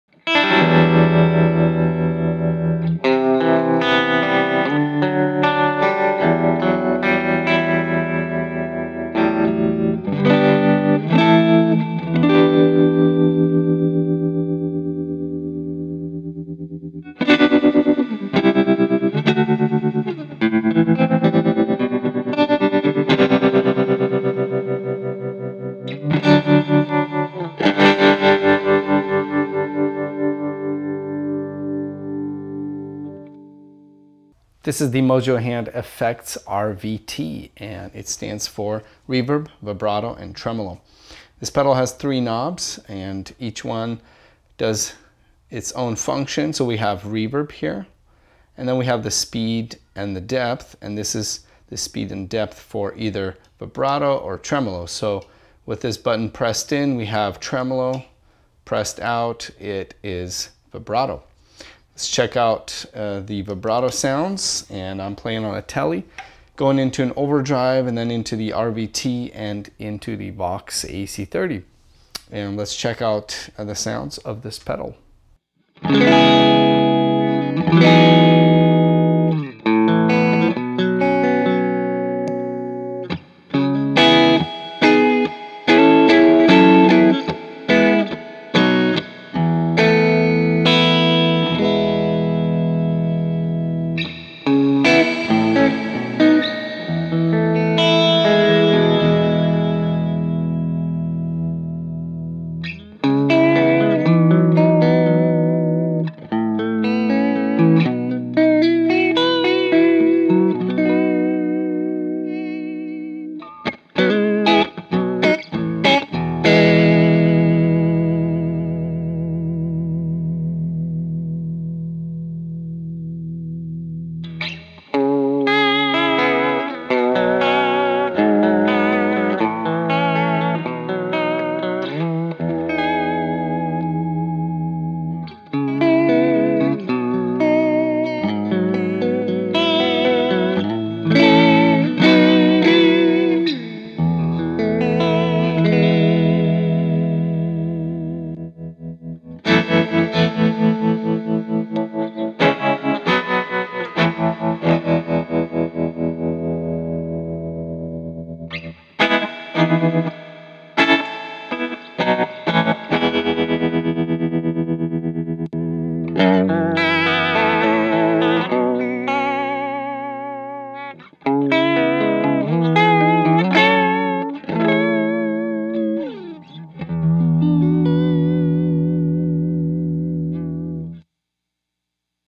Mojo Hand FX RVT
Find great deals on this pedal on Reverb . com

Signal Chain, Fender Telecaster into the Overdrive then RVT into a Vox AC30